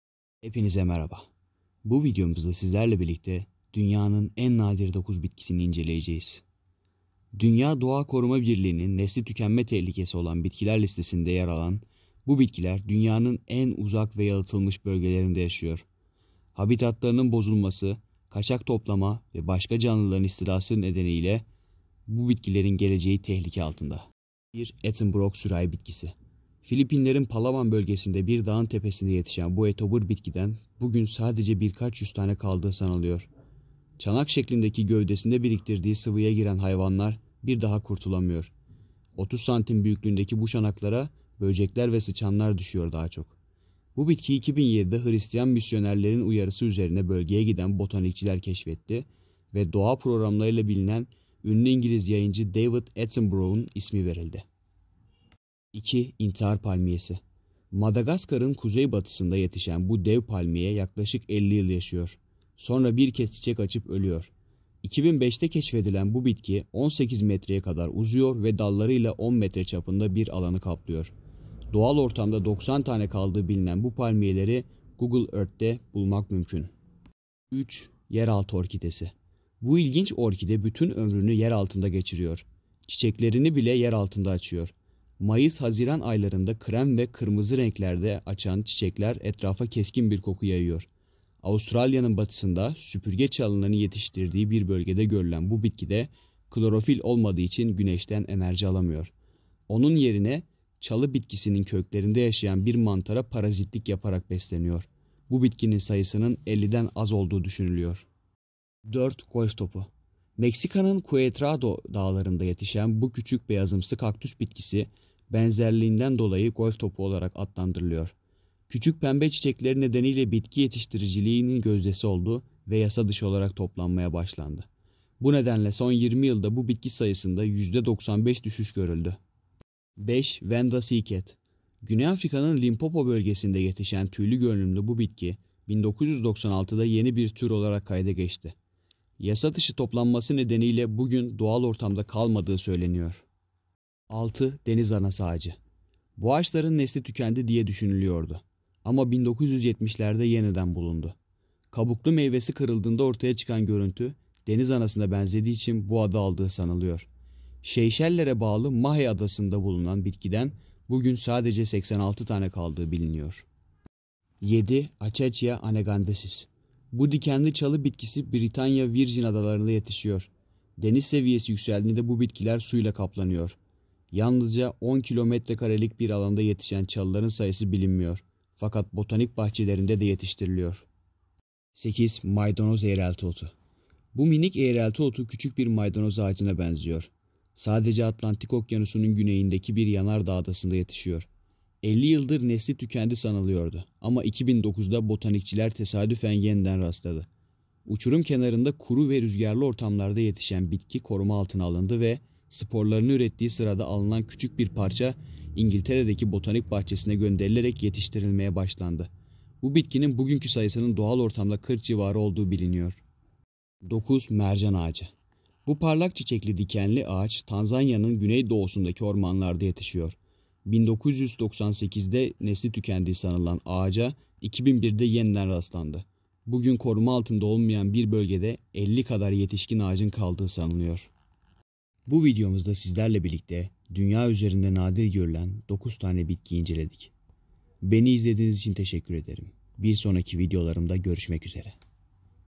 Dünyada En Az Bulunan 9 Bitki!
Bu videomuzda sizlerle birlikte Dünya üzerinde eşine çok az rastlanan ve hatta nesli tükenme tehlikesi altında olan 9 bitkiyi inceleyeceğiz.